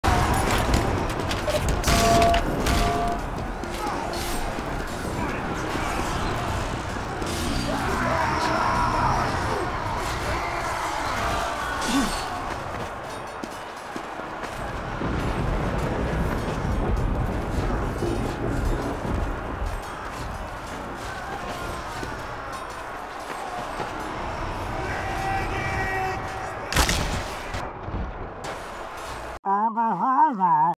Conscientious Objector №406 | TF2
Welcome to our latest TF2 Art Showcase video, featuring the Conscientious Objector! In this video, we'll be showcasing some of the best fan-made art from around the community, all centered around this iconic item.